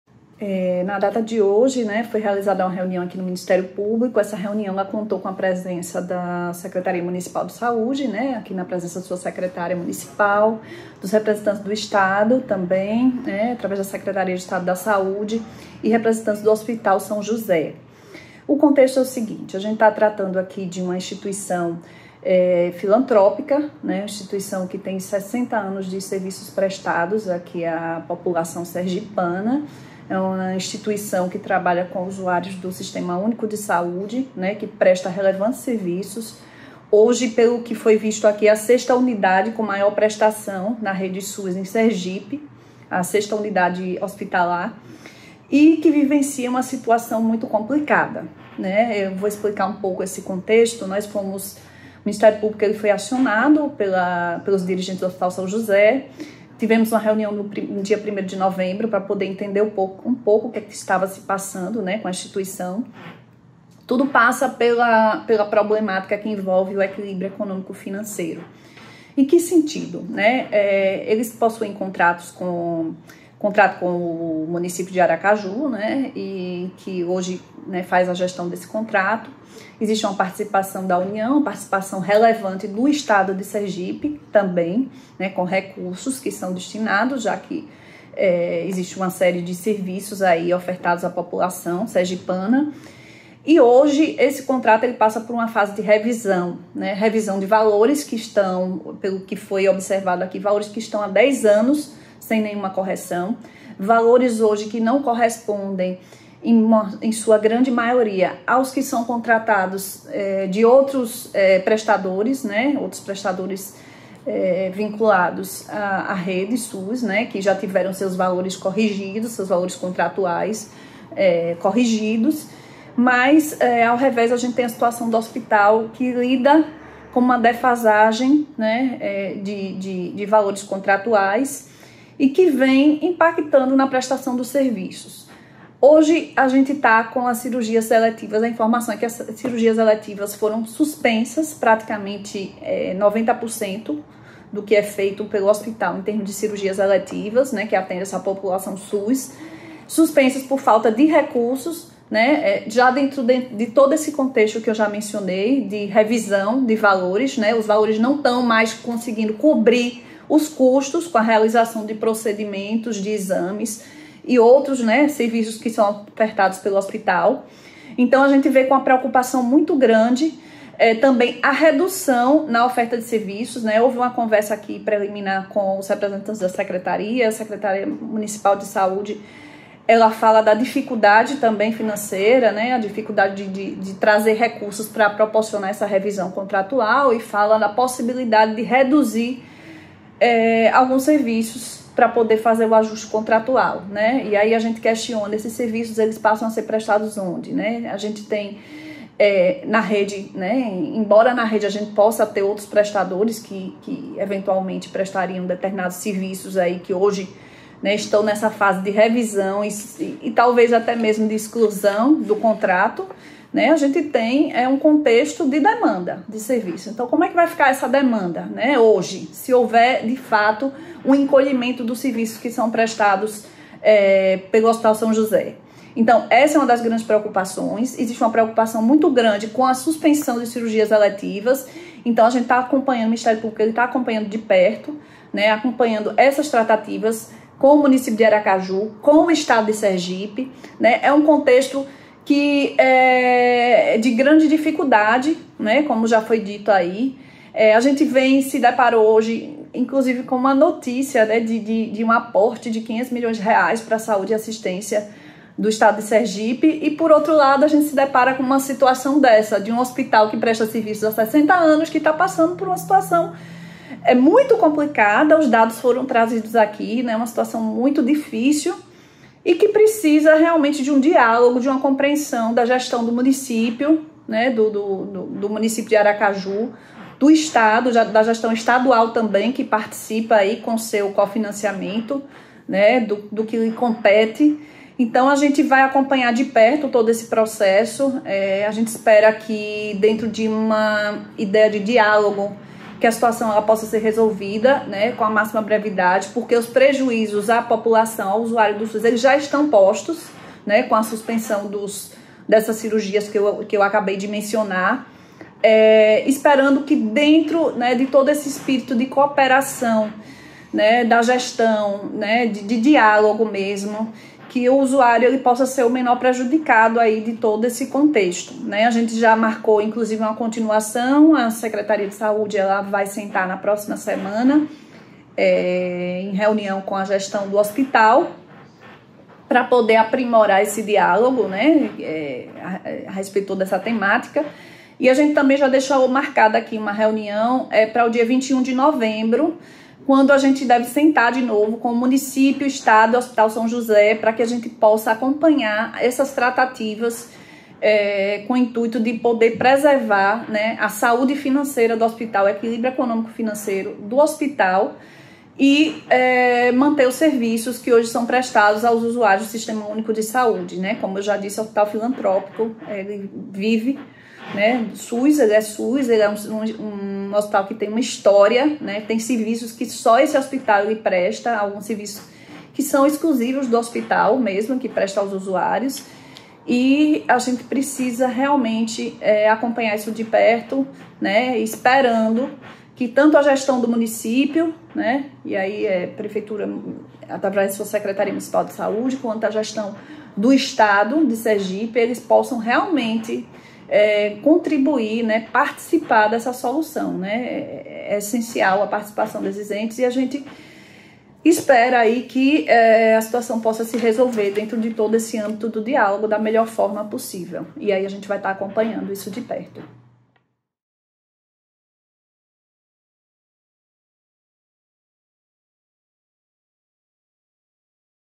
MPSE busca retomada de serviços no Hospital São José em audiência com Secretarias de Saúde
Diante da suspensão de serviços e assistência à população no Hospital São José, a exemplo das cirurgias eletivas, o Ministério Público de Sergipe, por meio da 9ª Promotoria de Justiça dos Direitos do Cidadão – especializada na Defesa dos Direitos à Saúde, realizou mais uma audiência para conduzir tratativas que possam solucionar o impasse. Além dos dirigentes do Hospital, também participaram, nesta ocasião, representantes das Secretarias de Saúde de Aracaju e do Estado.